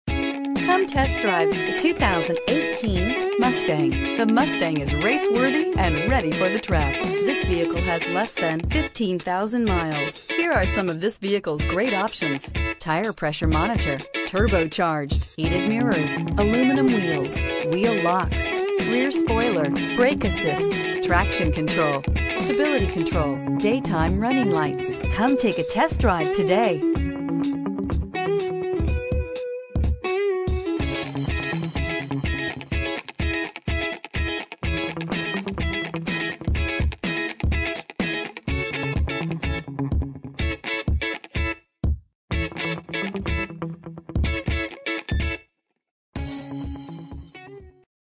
2018 Ford Mustang T1108191
2018 Ford Mustang EcoBoost Premium

CARFAX One-Owner. Clean CARFAX. Odometer is 34352 miles below market average! KBB Fair Market Range High: $26,666 KBB Fair Market Range Low: $24,255 2D CoupeRuby Red 2018 Ford Mustang EcoBoost Premium RWD 10-Speed Automatic EcoBoost 2.3L I4 GTDi DOHC Turbocharged VCTAwards:* 2018 KBB.com 10 Coolest Cars Under $25,000 * 2018 KBB.com 10 Most Awarded Brands * 2018 KBB.com Brand Image AwardsWe're happy to have you stop by anytime or call for an appointment today and EXPERIENCE THE DIFFERENCE in Ann Arbor Michigan.